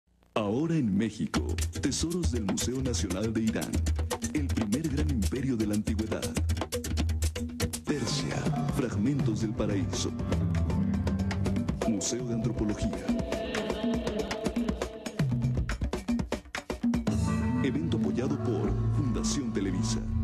Comerciales mexicanos: Exposicion Fundacion Televisa 2007
Exposición Persia fragmentos del paraíso en el Museo Nacional de Antropología a través de Fundación Televisa